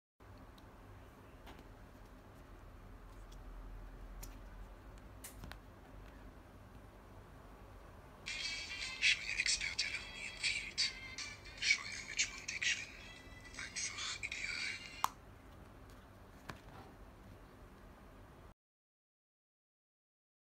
2.4inch video business card
A video business card is a compact and portable card that integrate a 2.4inch screen and custom design.